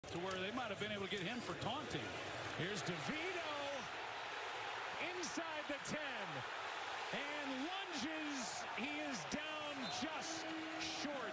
Tommy DeVito (Almost) rushes for a touchdown vs the Packers
new York Giants Tommy DeVito has a long run vs the Green Bay packers